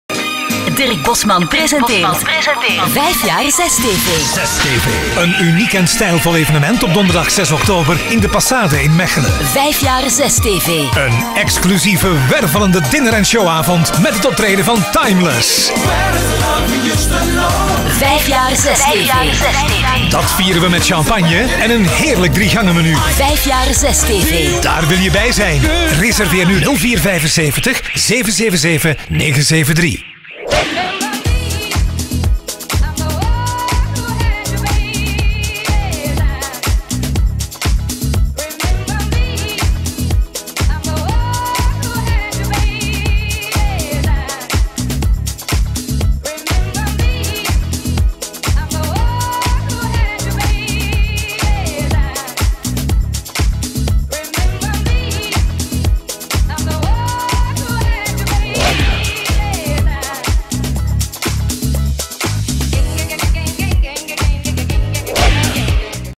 Feest 5 jaar 6TV
een voorproefje after movie van 5 jaar 6TV